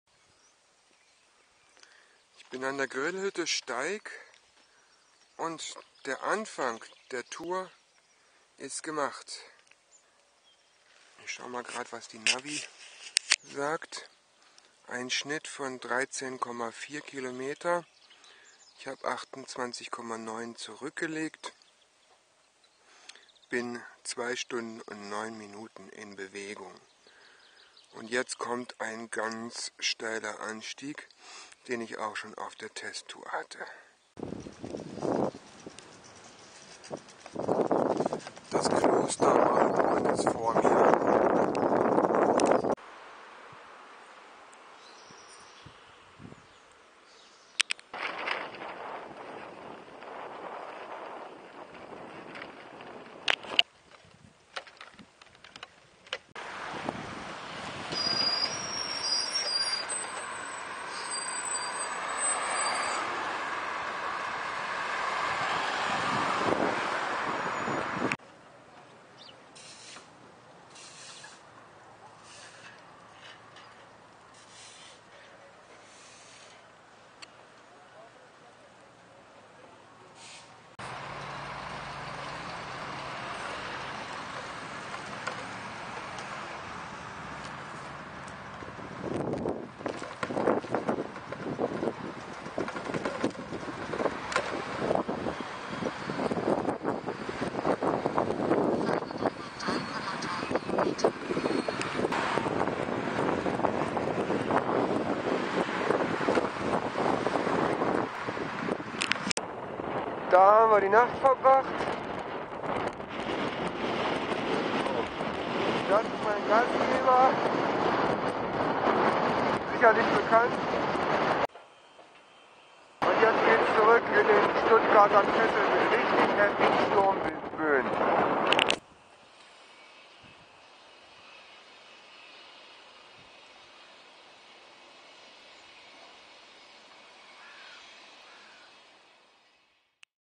OmaRadTour 1.Tag: Anfang durch Natur, Hektik in der Stadt und Übernachtung in Stuttgart
Motiviert geht es in den ersten Tag, stürmisch durch die erste Übernachtung draußen (Filmen war abends daher nicht mőglich).